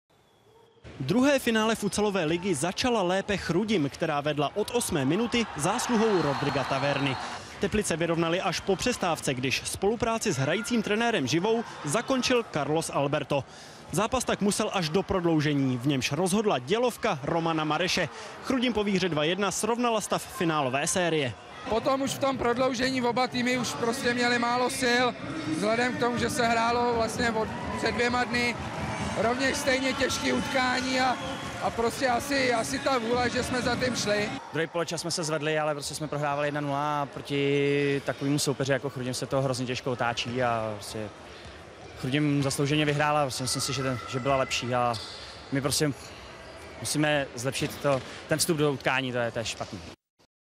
Reportáž ČT z druhého finále 2014/2015
Druhé finále Chance futsal ligy 2014/2015 ERA-PACK Chrudim - Balticflora Teplice